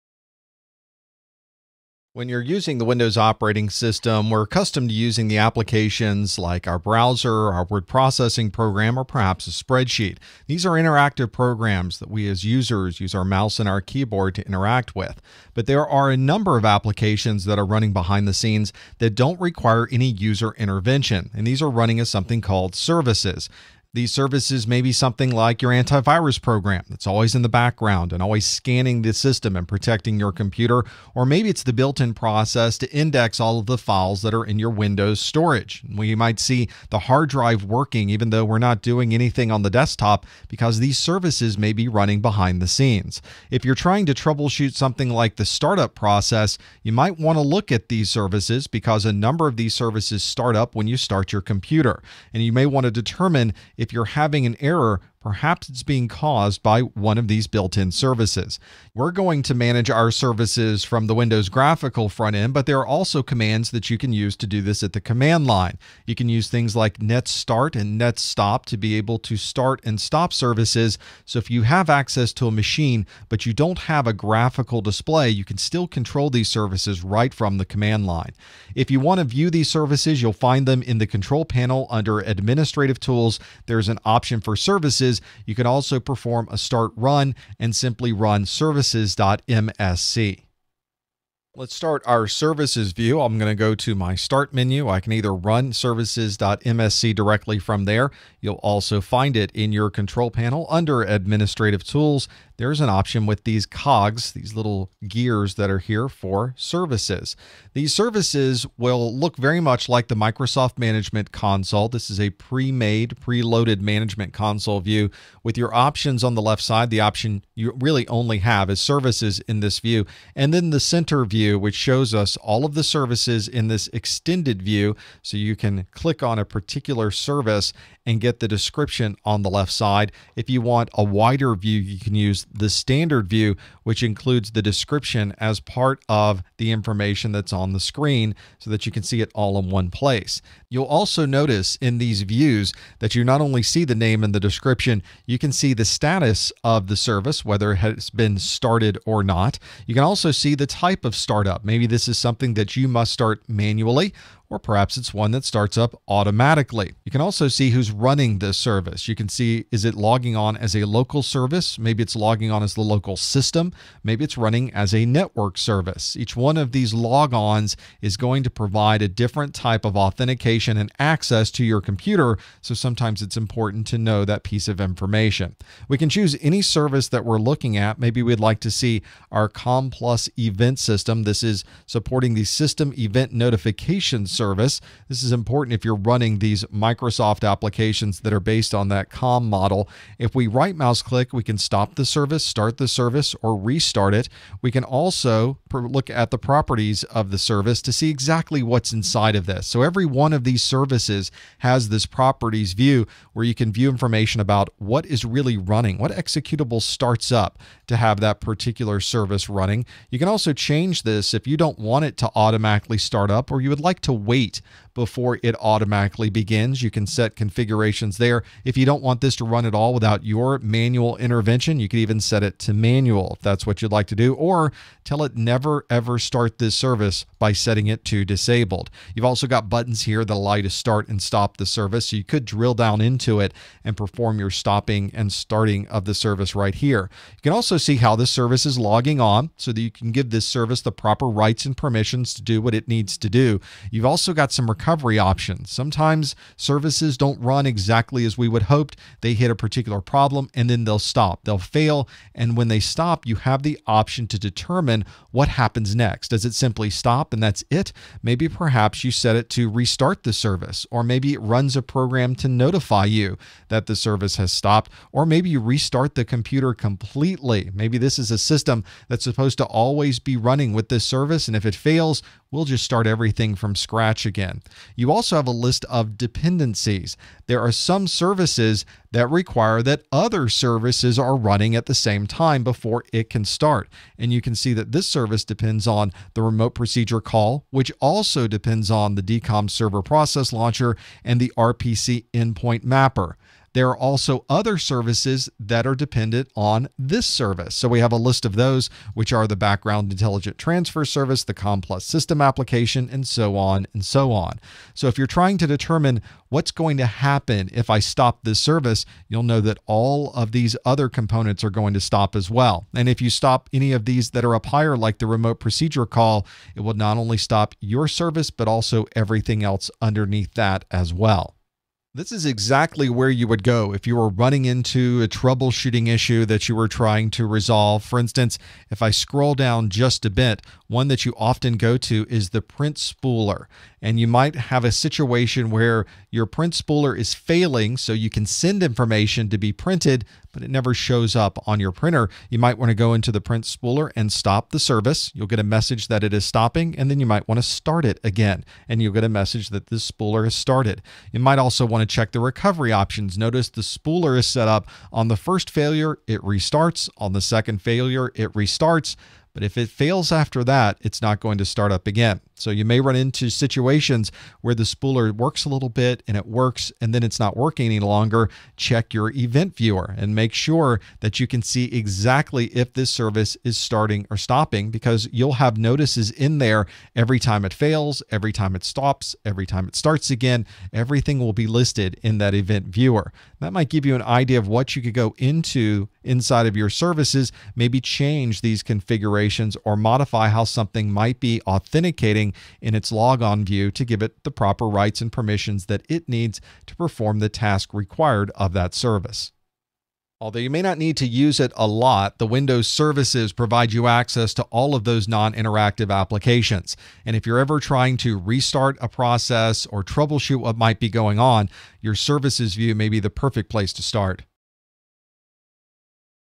Managing Windows Services - CompTIA A+ 220-802: 1.4
CompTIA has RETIRED the 220-800 A+ exam series!

The Windows Services console provides central management for all of your services. In this video, you'll learn how to start, manage, and configure service options in Windows.